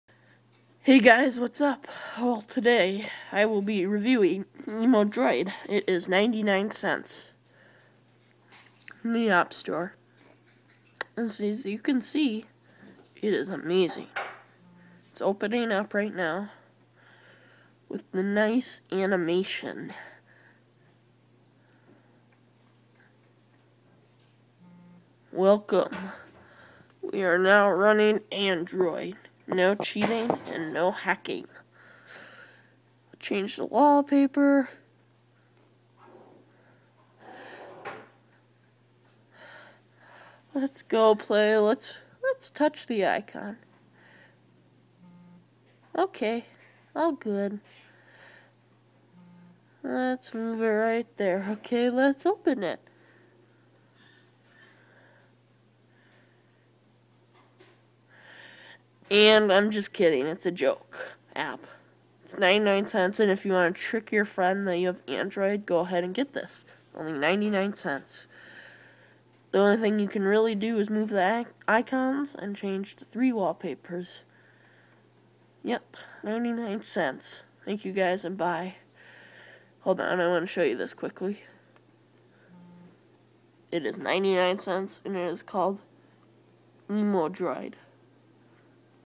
Emudroid app review
It android on iOS no jailbreak